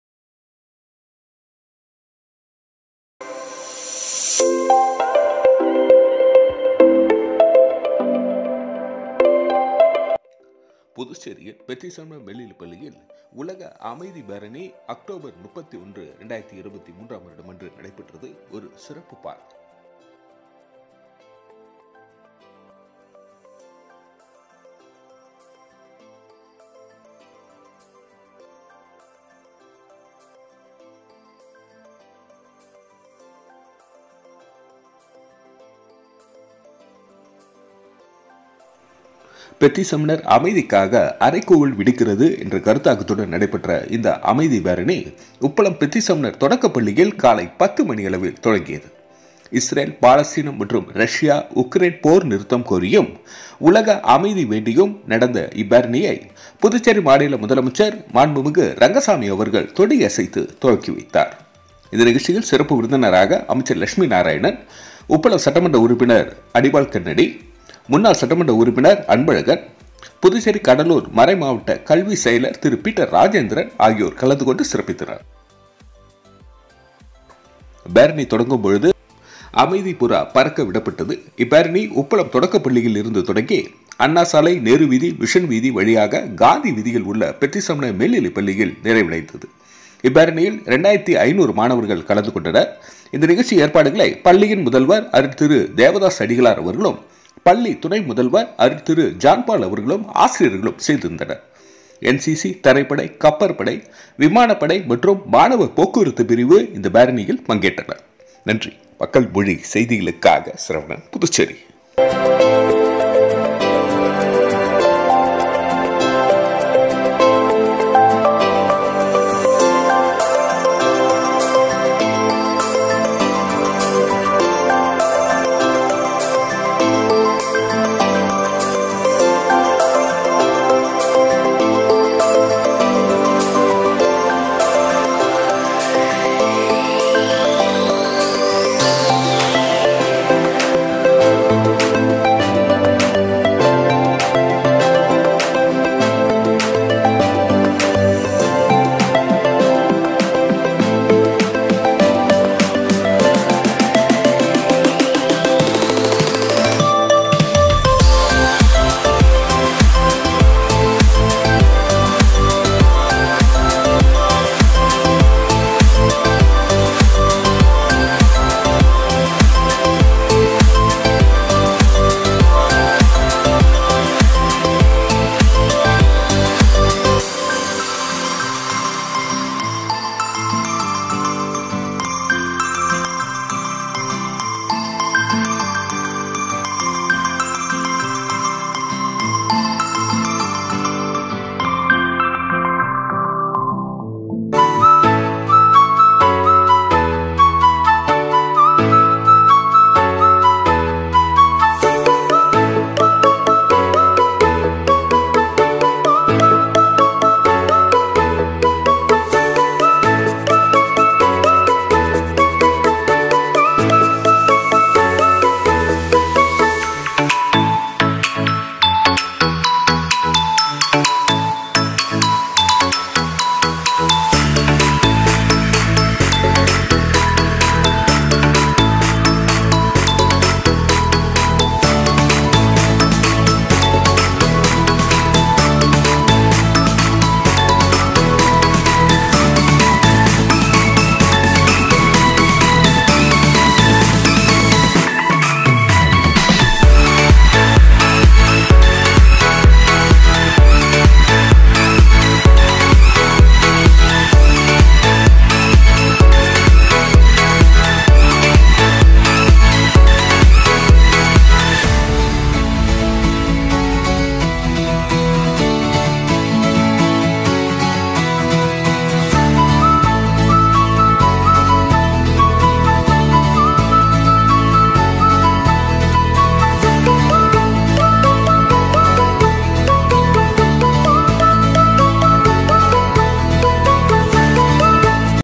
புதுச்சேரி பெத்தி செமினார் மேல்நிலைப்பள்ளியில் உலக அமைதிப் பேரணி 31.10.2023 அன்று நடைபெற்றது.
'பெத்தி செமினார் அமைதிக்காக அறைகூவல் விடுக்கிறது' என்ற சுருத்தாக்கத்துடன் நடைபெற்ற இந்த அமைதிப் பேரணி, உப்பளம் பெத்தி செமினார் தொடக்கப்பள்ளியில் காலை 10.00 மணியளவில் தொடங்கியது.

 இஸ்ரேல் பாலஸ்தீனம் மற்றும் ரஷ்யா - உக்ரேன் போர் நிறுத்தம் கோரியும், உலக அமைதி வேண்டியும், நடந்த இப்பேரணியைப் புதுச்சேரி மாநில முதலமைச்சர் மாண்புமிகு ந. ரங்கசாமி அவர்கள் கொடியசைத்துத் தொடங்கி வைத்தார்.

இந்நிகழ்ச்சியில் சிறப்பு விருந்தினர்களாக மாண்புமிகு கே. லட்சுமி நாராயணன் பொதுப்பணித்துறை அமைச்சர் அவர்களும், உப்பளம் சட்டமன்ற உறுப்பினர் மாண்புமிகு அனிபால் கென்னடி அவர்களும், முன்னாள் சட்டமன்ற உறுப்பினர் உயர்திரு அ.அன்பழகன் அவர்களும், புதுச்சேரி-கடலூர் மறை மாவட்டக் கல்விச் செயலர் திரு பீட்டர் ராஜேந்திரன் அவர்களும், கலந்து கொண்டு சிறப்பித்தனர்.

பேரணி தொடங்கும் போது அமைதிப்புறா பறக்க விடப்பட்டது. இப்பேரணி உப்பளம் தொடக்கப்பள்ளியில் இருந்து தொடங்கி அண்ணா சாலை, நேரு வீதி, மிஷன் வீதி வழியாக காந்தி வீதியில் உள்ள பெத்தி செமினார் மேல்நிலைப்பள்ளியில் நிறைவடைந்தது.

இப்பேரணியில் 2500 மாணவர்கள் கலந்து கொண்டனர். இந்நிகழ்ச்சி ஏற்பாடுகளை பள்ளியின் முதல்வர் அருட்திரு அ.தேவதாஸ் அடிகளார் அவர்களும், பள்ளி துணை முதல்வர் அருட்திரு ஜான் பால் அவர்களும் ஆசிரியர்களும் செய்திருந்தனர்.

NCC,  தரைப்படை, கப்பற்படை, விமானப் படை மற்றும் மாணவர் போக்குவரத்து பிரிவு இந்தப் பேரணியில் பங்கேற்றனர்.

Makkal mozhi